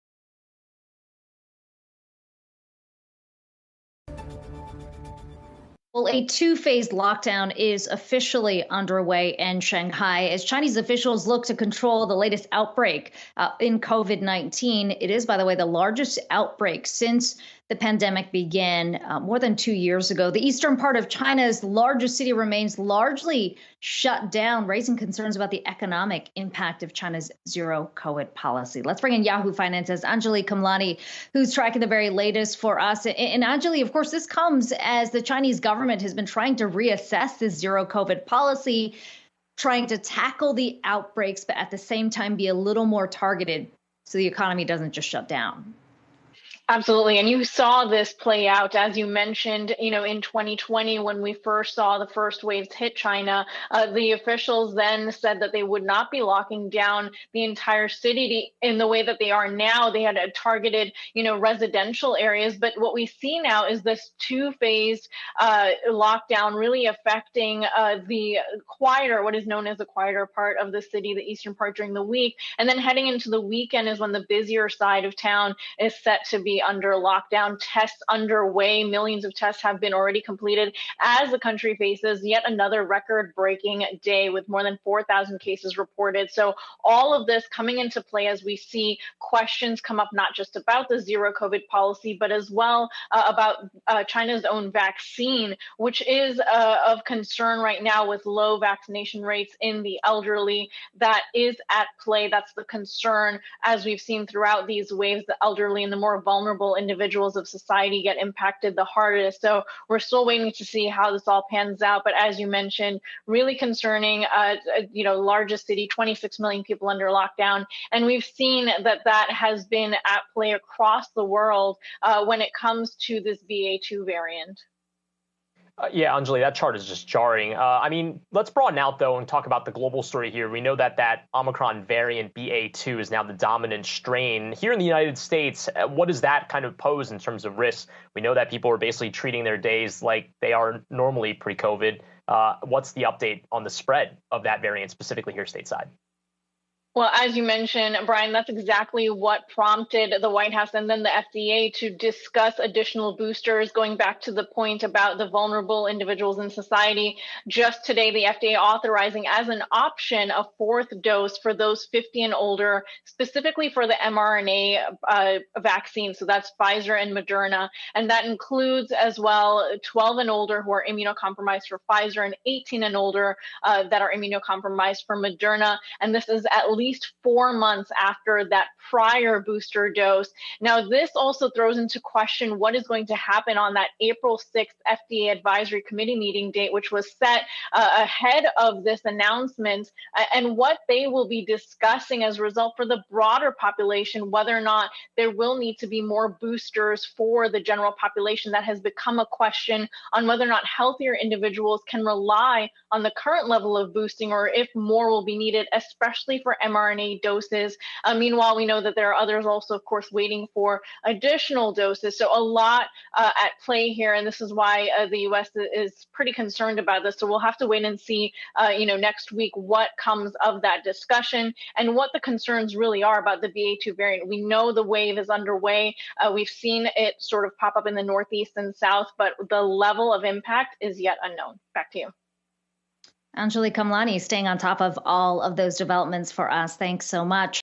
China COVID-19 outbreak: 26 million people under lockdown in Shanghai
Yahoo Finance's Anjalee Khemlani explains the latest news on the coronavirus and the extent of Shanghai's COVID lockdown.